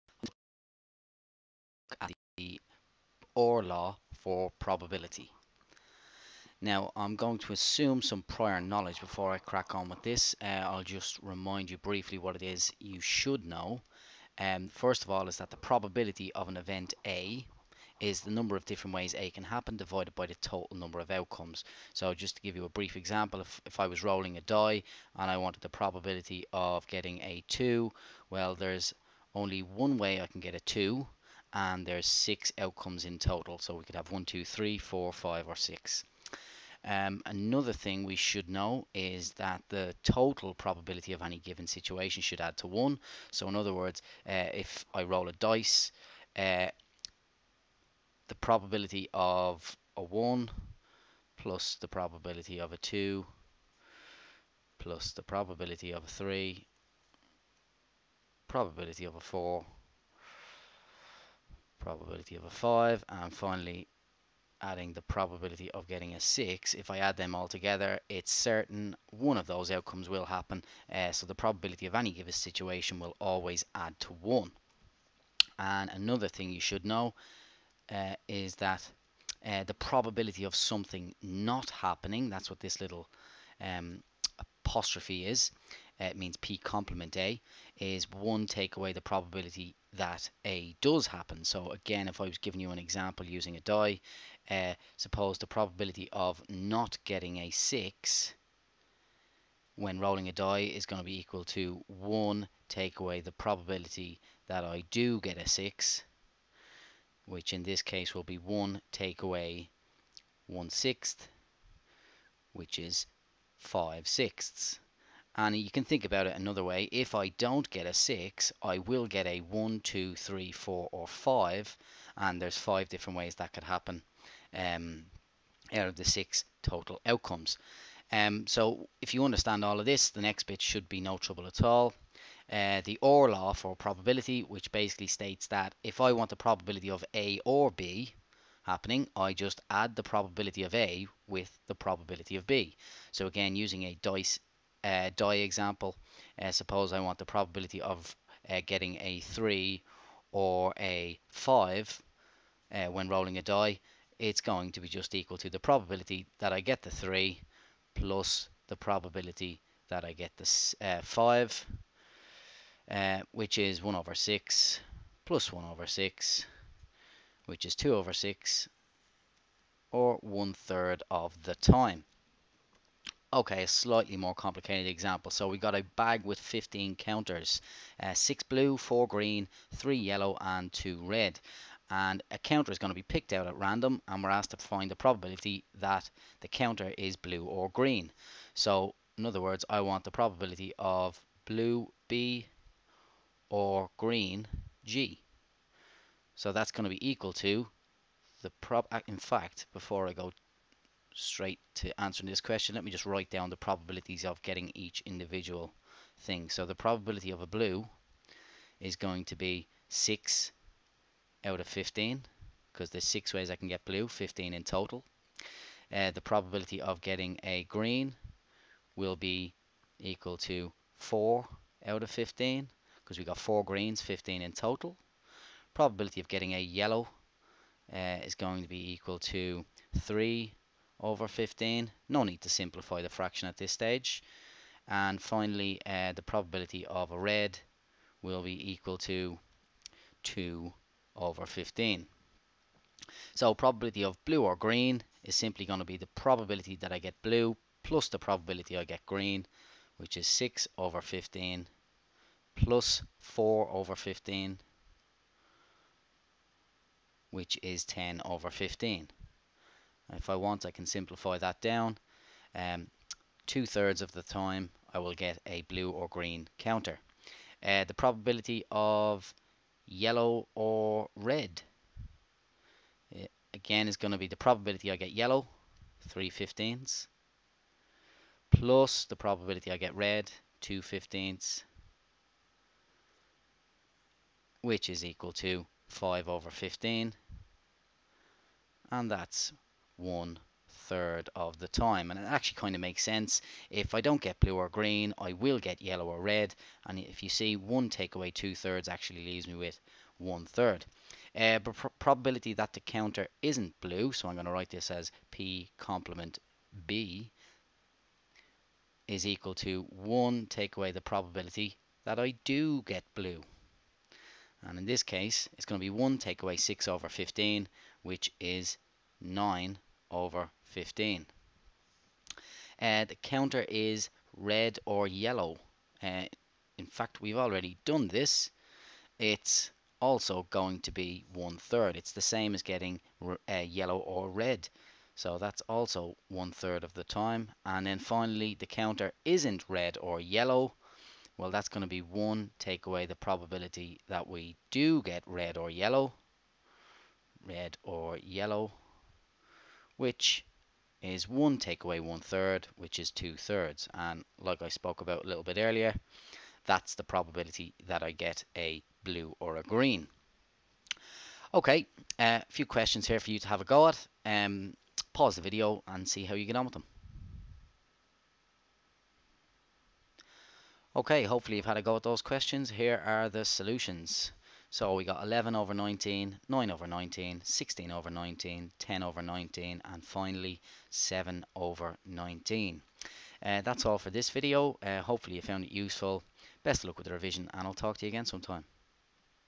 Probability (Or Law)
How to use the or law for probability